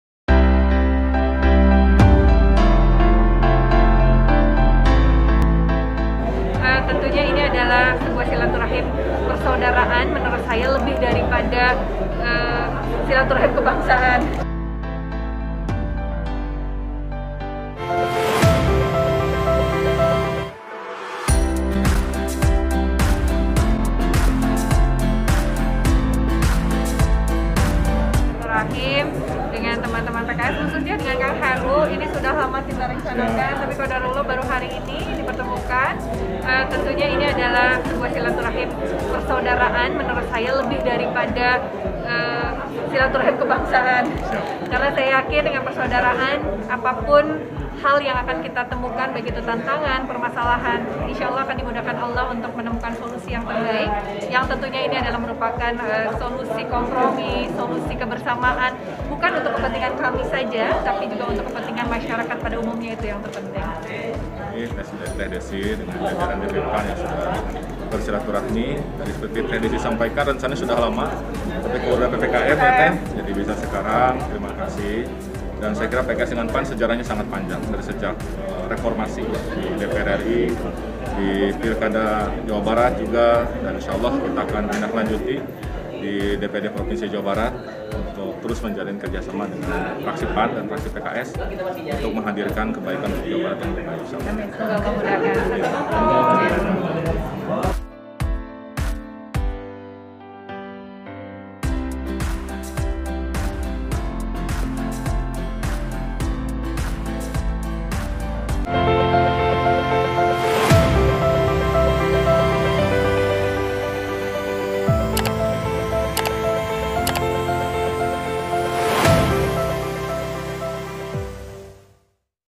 Silaturahim Kebangsaan PKS-PAN Jawa Barat, Desy Ratnasari : Serasa Datang ke Rumah Saudara
PKS Jawa Barat menerima kunjungan DPW PAN Jabar dalam acara Silaturahim Kebangsaan PKS-PAN yang berlangsung di DPW